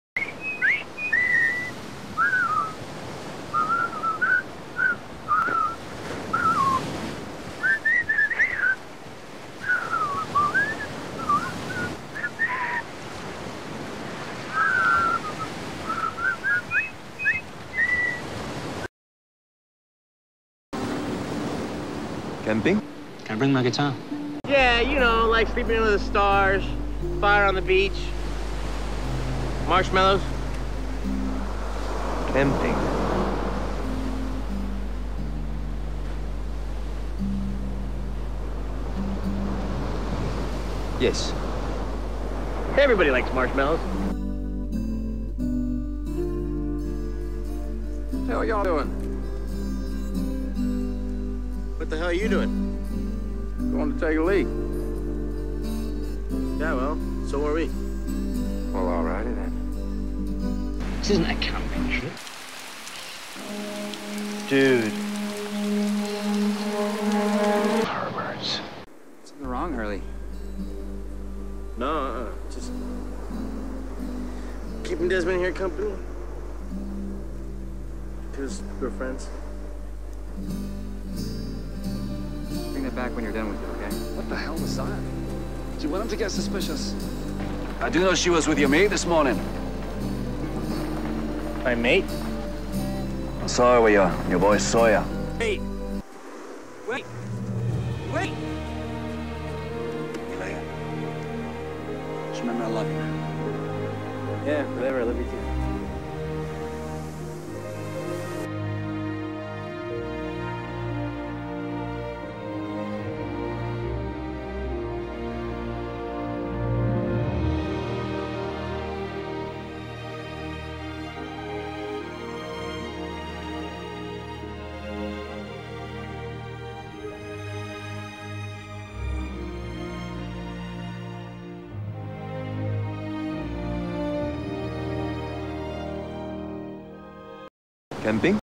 Chesmondo
In this SEQUEL to the CRITICALLY ACCLAIMED "CHESMOND", Charlie, Dez, Hugo, and Jin, go on a "camping" trip.  But there is more to this "trip" than meets the eye. . . .

R.I.P.  Charlie Pace.